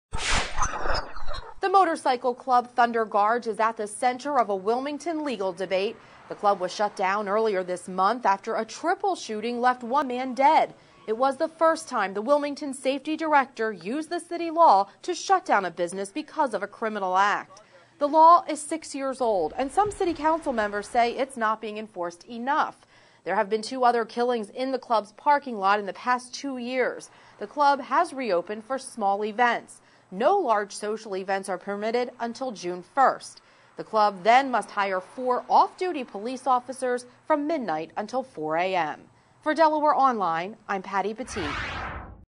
Delaware Online News Video: Thunderguards at center of debate over Wilm. law
A triple shooting that left one man dead outside the Thunderguards Motorcycle Club earlier this month led the city's public safety director to shut down the group's social hall on the spot.